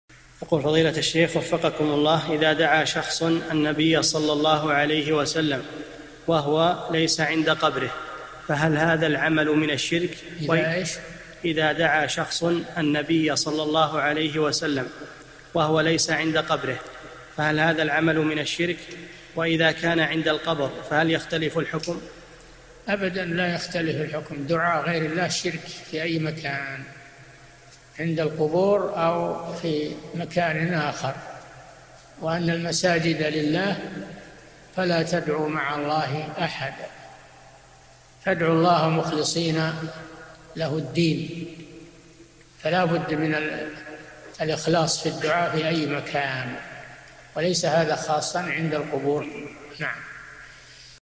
[19 /1350] إذا دعا شخص النبي ﷺ، وهو ليس عند قبره، يعد من الشرك وهل هناك فرق إذا كان عند قبره؟
[19 /1350] السؤال: إذا دعا شخص النبي ﷺ، وهو ليس عند قبره، فهل هذا العمل من الشرك، وإذا كان عند القبر، فهل يختلف الحكم؟
 الجواب: أبدًا لا يختلف الحكم، دعاء غير الله شرك في أي مكان، عند القبور، أو في مكان آخر، ﴿وَأَنَّ الْمَسَاجِدَ لِلَّهِ فَلَا تَدْعُوا مَعَ اللَّهِ أَحَدًا 18﴾ [الجن: 18]، ﴿فَادْعُوا اللَّهَ مُخْلِصِينَ لَهُ الدِّينَ 14﴾ [غافر: 14]، فلا بد من الإخلاص في الدعاء في أي مكان، وليس هذا خاصًّا عند القبور.
للاشتراك في قناة الشيخ صالح الفوزان
أسئلة وأجوبة من شرح قاعدة جليلة ( 1350 ) مقطع

للاستفسار والإقتراحات والدعم
رئيس الفريق
لمشروع كبار العلماء